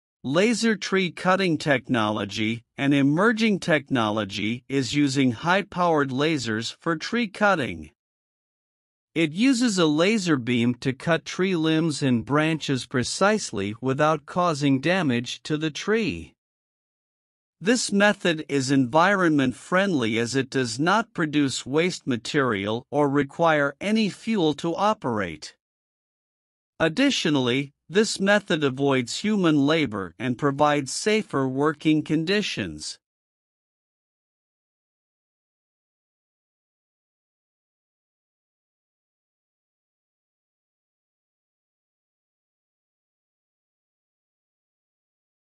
laser tree cutting advance technology
Laser tree cutting technology: An emerging technology is using high-powered lasers for tree-cutting. It uses a laser beam to cut tree limbs and branches precisely without causing damage to the tree. This method is environment friendly as it does not produce waste material or require any fuel to operate. Additionally, this method avoids human labor and provides safer working conditions.

These are some of the advanced technologies for cutting trees. While choosing them, users should consider factors such as environmental impact, cost, and efficiency, among other things.